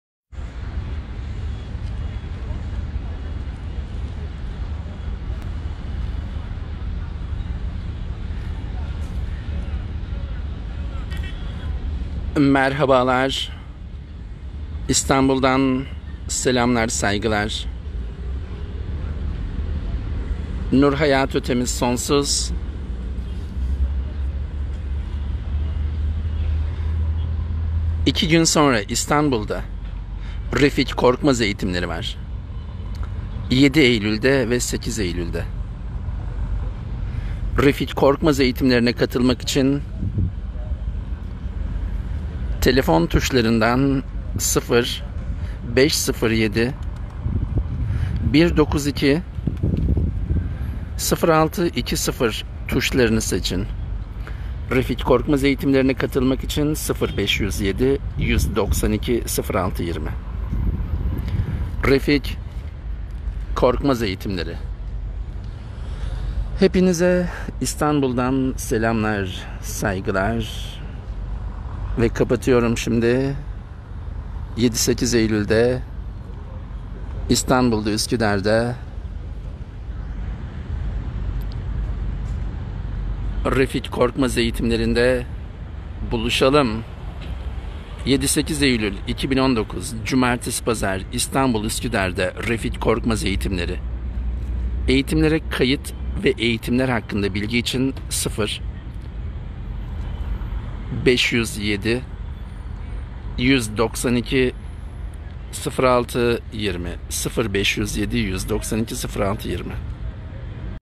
Refik Korkmaz 7 ve 8 Eylül İstanbul Eğitimleri var
7 - 8 Eylul 2019 İstanbul, 1 - 12 Eylul 2019 Jaksonville egitimini Refik Korkmaz veriyor.

“Refik Korkmaz Bilinçaltı Dili Mucizeler EĞİTMENLİK EĞİTİMİ” Eylül 2018 itibariyle başladı.
14 Eylül EĞİTMENLİK Eğitimi; uygulama içeren bu eğitim 1. ve 2. modül eğitimleri verme hakkı kazandırır.

Refik Korkmaz
Bilinçaltı Dili ve Mucizeler Sistemin Kurucusu